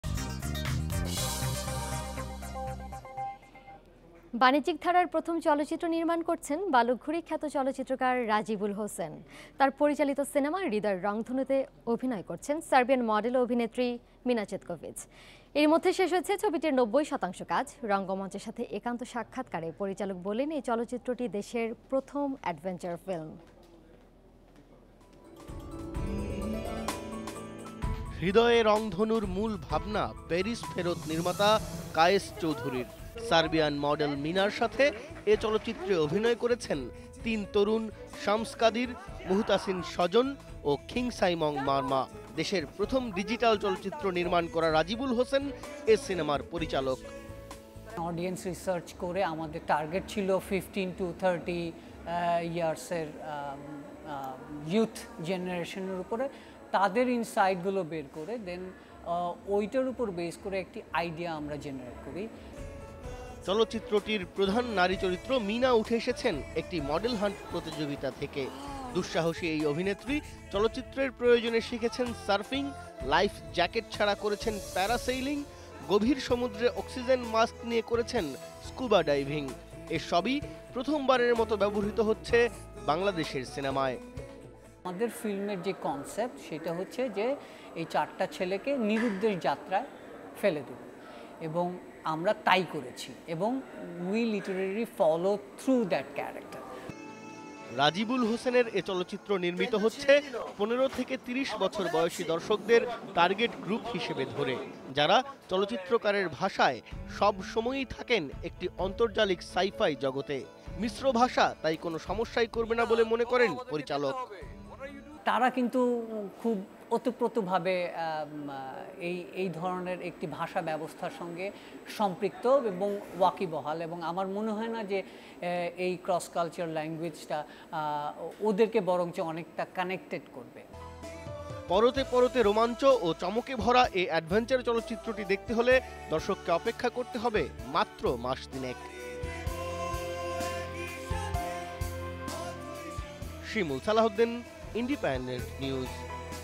Life in Rainbow - হৃদয়ের রংধনু | Bangladeshi Movie | TV News
First Look: Life in Rainbow - হৃদয়ের রংধনু movie (Bangladesh) Reported By Shimul Salahuddin, Independent TV

Synopsis:

It’s is a story of Four aspiring youth (3 boys & 1 girl). They are friends. They have everything in their life - money, education, and social status… EVERYTHING…except the LIFE within the LIFE! One day their life turns out to be misfortunes. They start thinking; Who they are? Where they are?

Suddenly, They got a call from a mysterious character as HE (the mysterious character) knew everything about them (Mina, Shams, Shojon & Khing); their desire, expectations, inner calls etc. HE offered them a trip, “If you are dare enough to take any challenges you can join a trip, a trip to unknown destination!!!” They never thought this journey would make them a new person with enlightens of inner knowledge, skills and personality.

হৃদয়ের রংধনু … (Life in Rainbow)

Director: Razibul Hossain
Production: AIMC
Genre: Reality Fiction (Adventure)
Country: Bangladesh
Duration: 120 min.

Team:

Direction: Razibul Hossain
Story: Kayes Chawdhury
Director of Photography: Razibul Hossain
Editing: Razibul Hossain

Chief Assistant Director: Dinar Hasan
Art Director: Rayhan Shoshi

Man Cast:
Mina Petkovic
Muhtasim Shojon
Khing Sai Mong
Shams Kadir